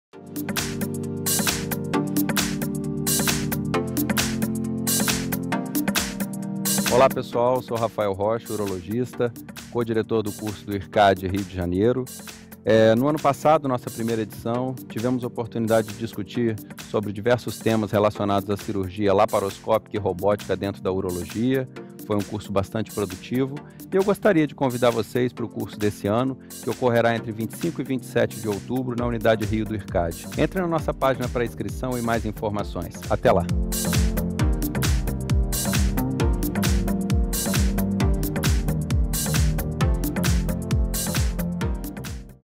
Vem aí: Cirurgia Urológica - Unidade Rio de Janeiro
Raphael Rocha é co-diretor do Curso de Cirurgia Urológica da Unidade Rio de Janeiro e convida para o curso, que vai ser realizado de 25 a 27 de Outubro de 2018.